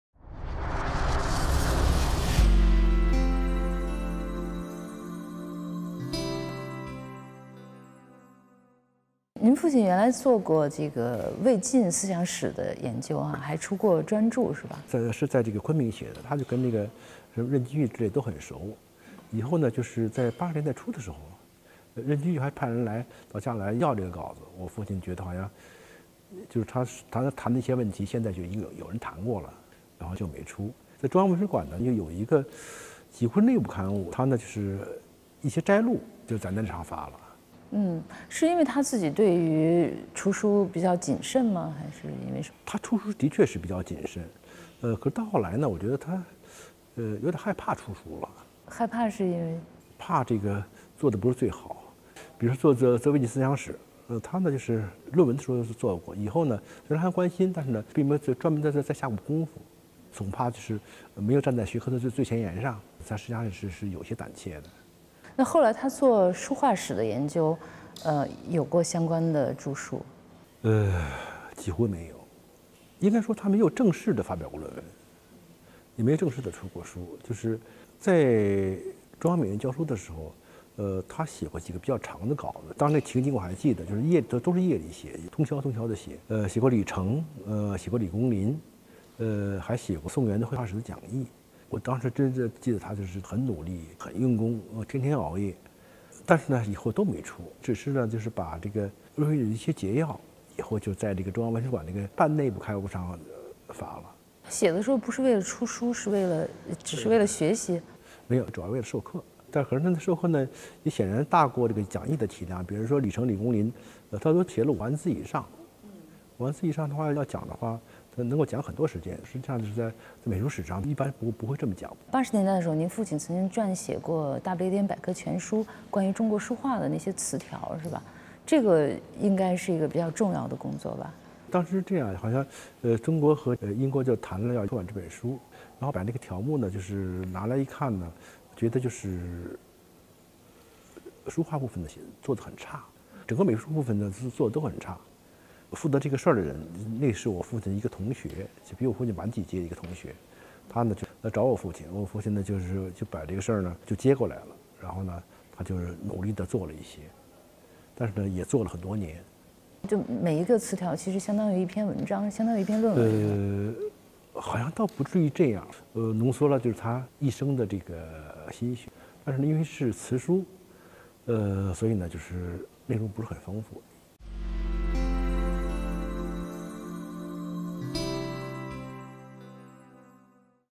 清华教授尚刚：父亲尚爱松曾为授课写5万字以上的讲义
人文清华短视频：清华大学美术学院教授尚刚讲述自己的父亲，中国著名美术史家尚爱松先生写作不为出书只为授课，曾编撰《简明大不列颠百科全书》。